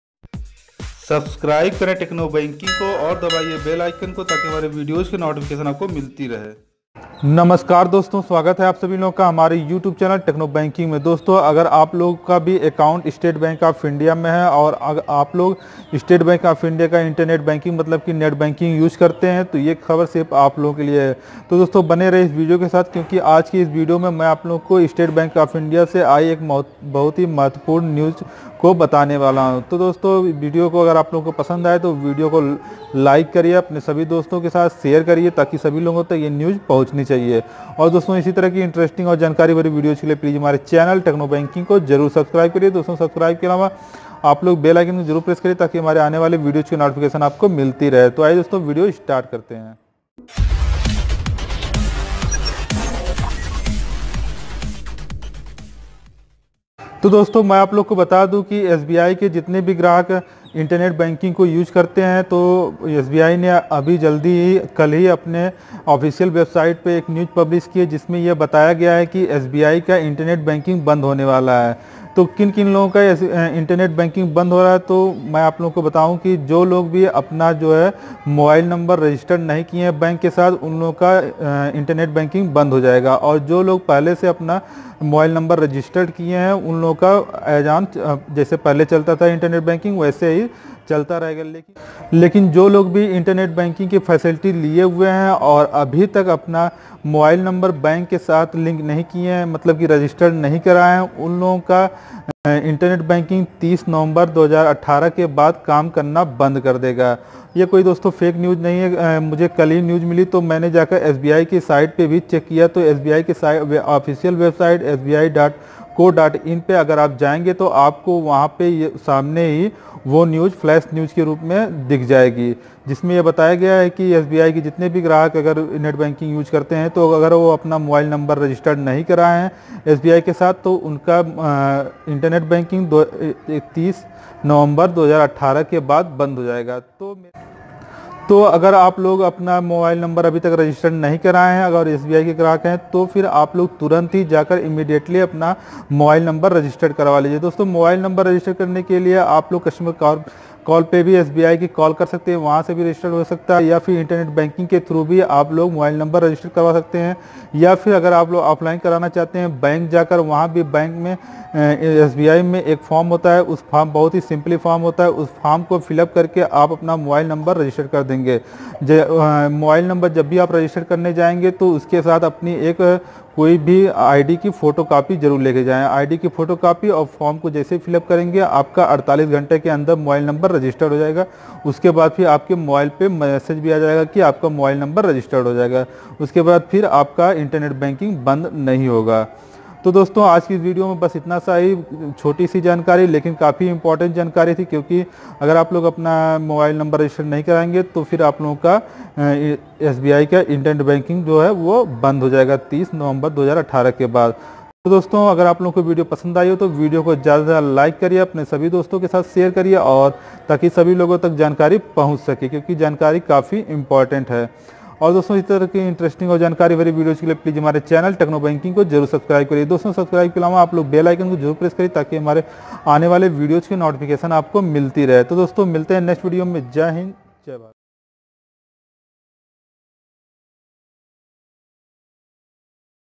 State Bank of India New Announcement:: SBI Internet Banking blocked after 30 November 2018
Namskar Dosto
           Swagat hai aap sabhi logo ka hamare youtube channel TechnoBanking me. Dosto aaj ke is video me hamne State Bank of India ke new announcement ke bare me bataya hai jisme yeh batya gaya hai agar sbi ke jo customer internet banking service ko use kar rahe hai aur abhi tak apna mobile number bank ke sath registered nahi karwaye hai to unki internet banking service 30 November ke bad se band ho jayega.

username- mauryapk

username- @pkmaurya123

username- @technobankingbypraveen